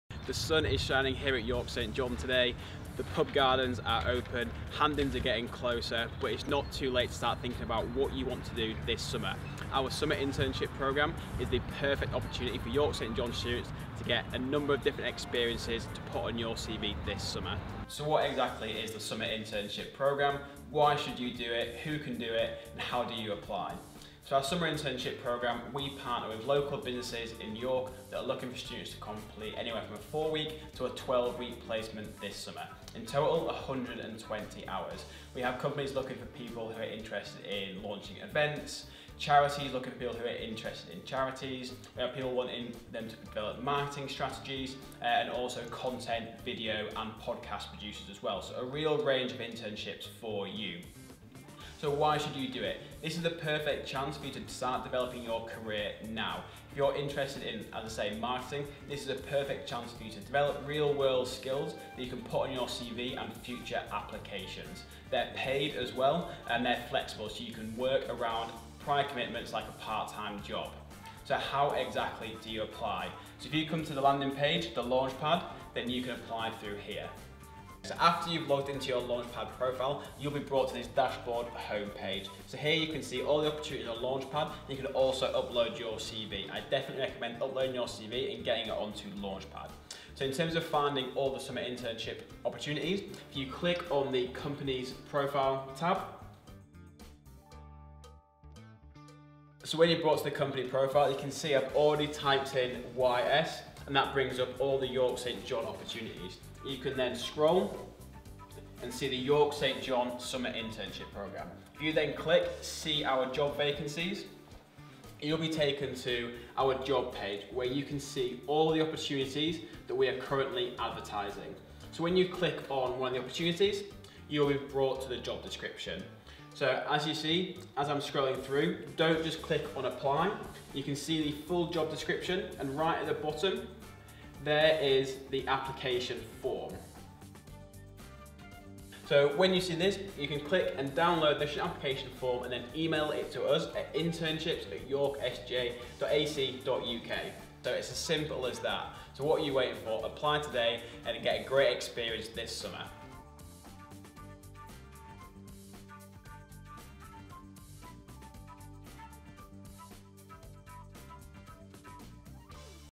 LaunchPad | Summer Internship Programme
Watch this video to find out about our Summer Internship Programme 2021!

You will find out why you should do a Summer Internship and how you can apply!